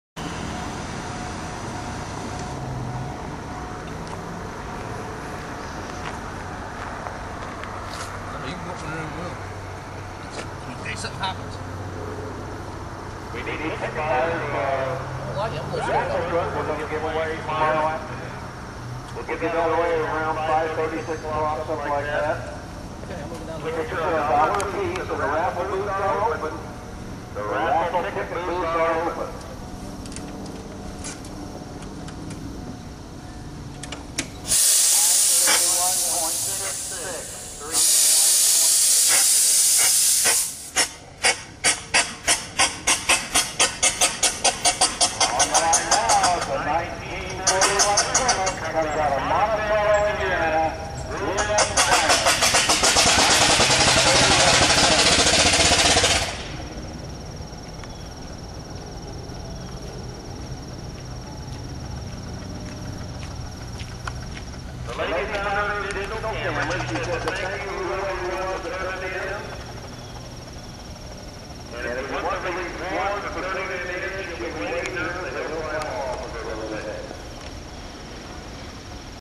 2009 Newport Hill Climb Stanley Steamer
Getting the car fired up for the run.